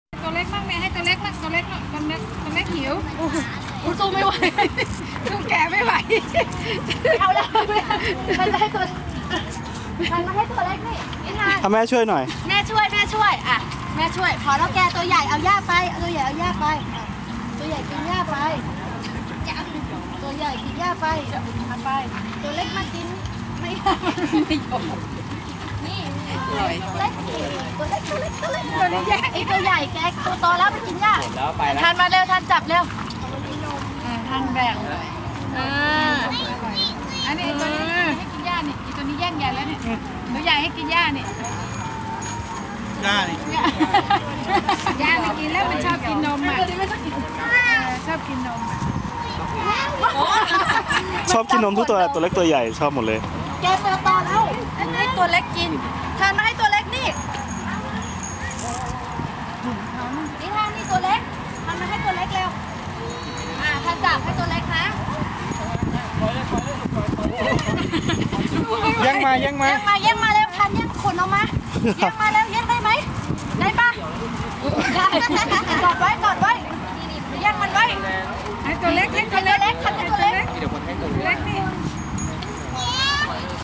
20140114 นิทานให้นมแกะที่ Swiss Sheep Farm อีกแล้ว
ไปเที่ยวที่นี่อีกครั้งก่อนกลับบ้าน คราวนี้แกะมารุมเพียบเลย